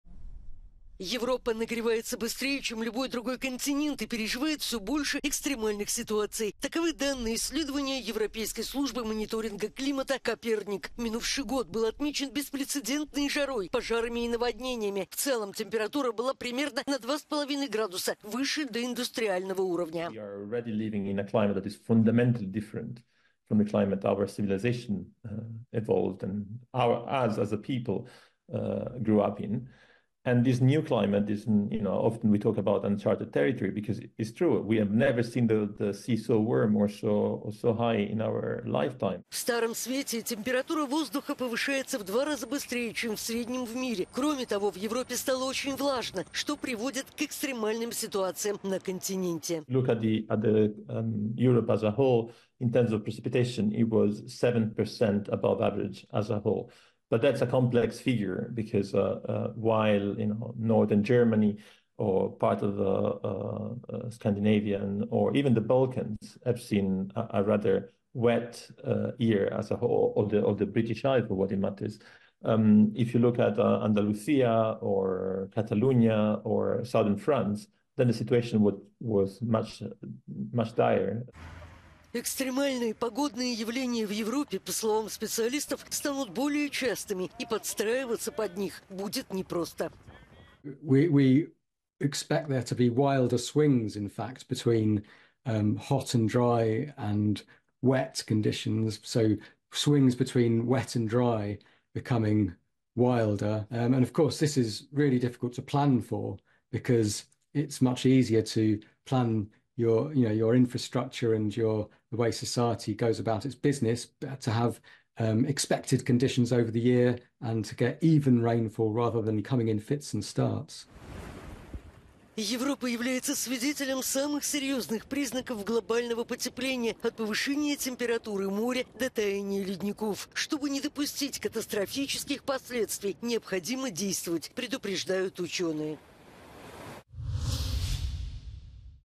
Изменение климата: в Европе становится жарко и влажно
Европа стала свидетелем самых серьезных признаков глобального потепления: от повышения температуры моря до таяния ледников.